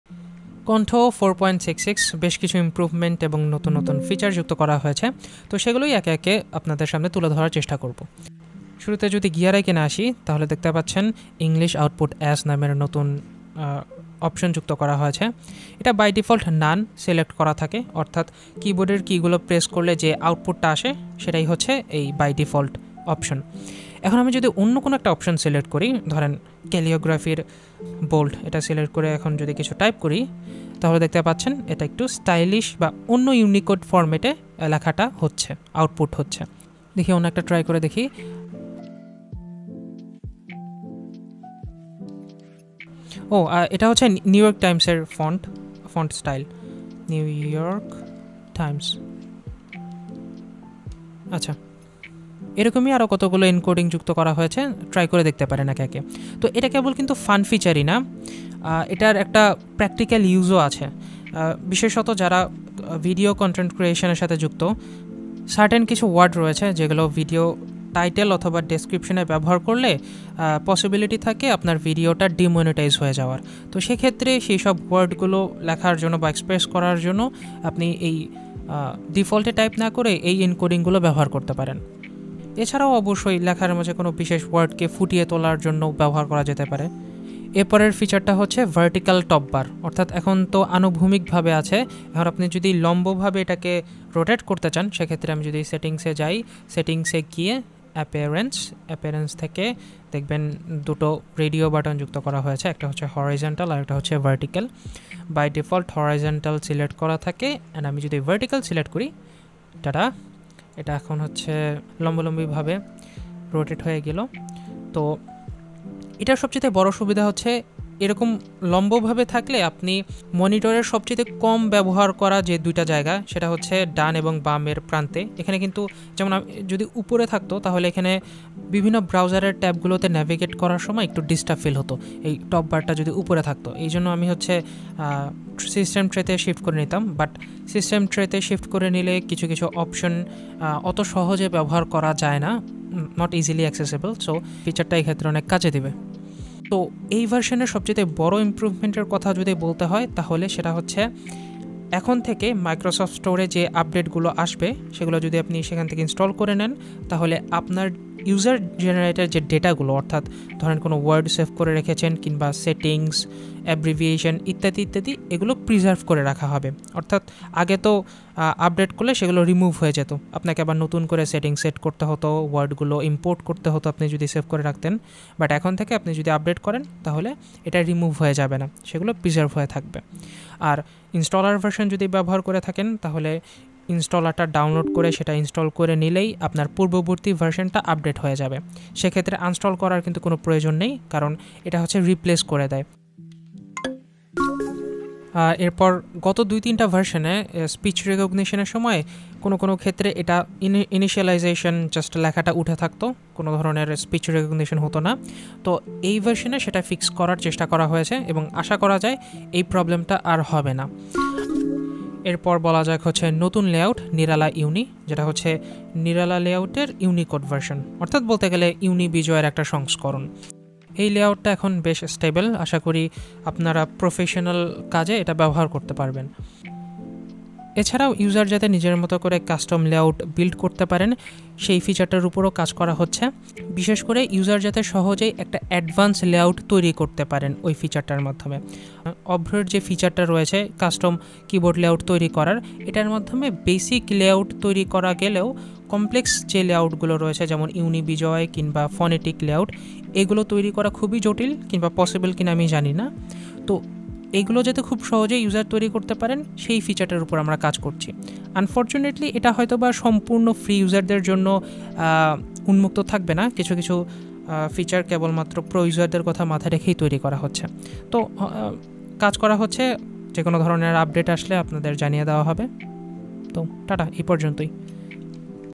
Update 4.66 | 𝓕𝓾𝓷 𝓕𝓮𝓪𝓽𝓾𝓻𝓮𝓼 | ℕ𝕄𝕊 𝕂𝕠𝕟𝕥𝕙𝕠 𝙺𝚎𝚢𝚋𝚘𝚊𝚛𝚍
v4.66 Update:
1.From this version, updating from the MS app store will preserve user data.
2. Tried to fix some small bugs.
3. Faster speech recognition.
4. Added vertical top bar option for the pro version.